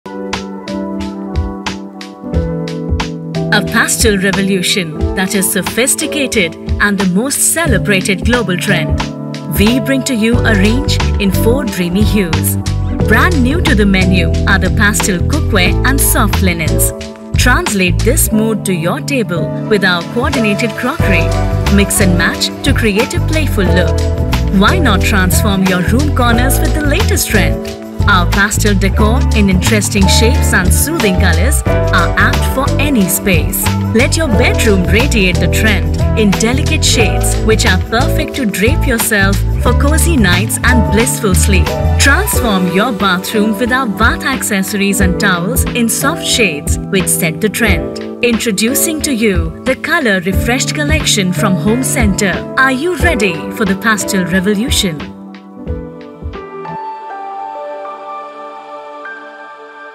Home Centre brings to you “Colour Refreshed Collection” A pastel revolution.
A pastel revolution that is sophisticated and the most celebrated global trend. Home Centre brings to you the “Colour Refreshed” collection in four dreamy hues. A curated range spread across crockery, kitchen and table linens, décor and furnishings. Shop the “Colour Refreshed’ collections at Home Centre stores or homecentre.com